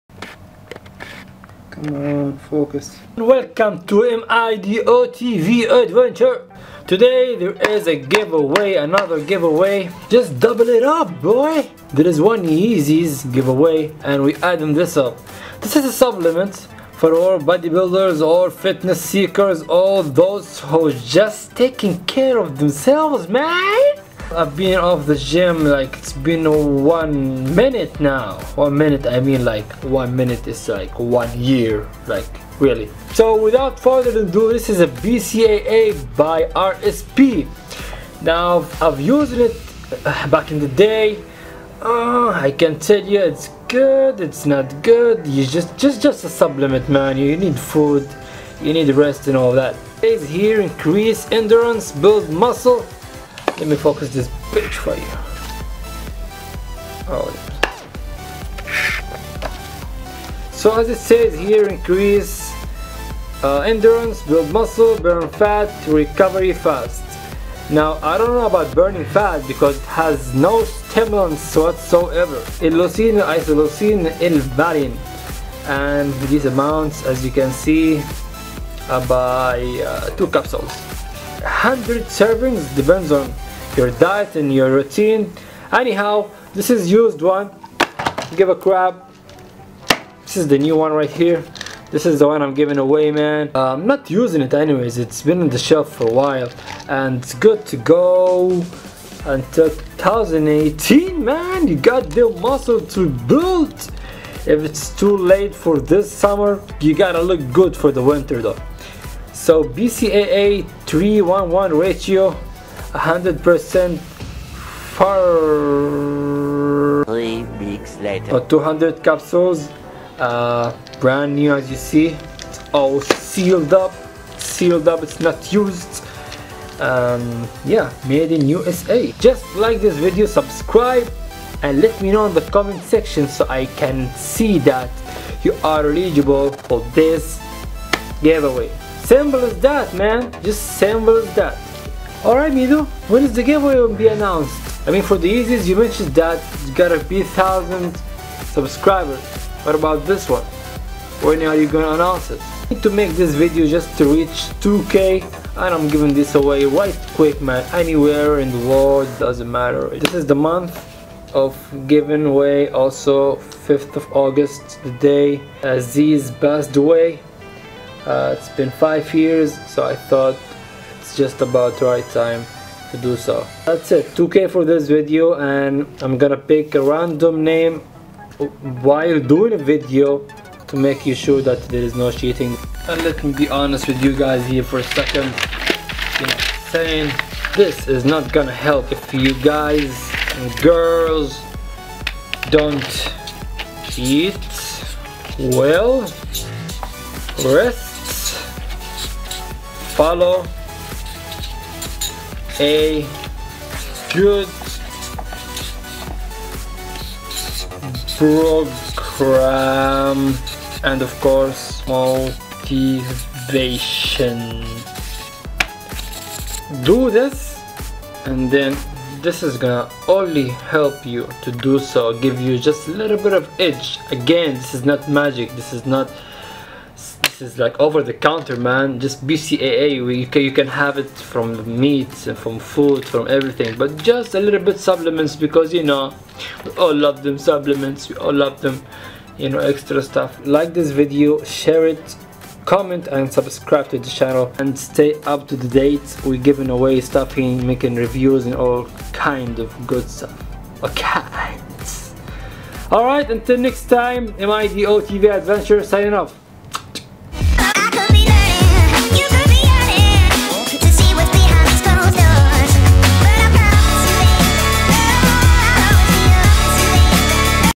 RSP BCAA Review & GIVEAWAY!! 2016
And we are back at it again, in Honor of Zyzz's Legacy i wanted to do this, may he Rest in Peace. you mirin? ;) FREE GIVE-AWAY easy and simple enter to win some.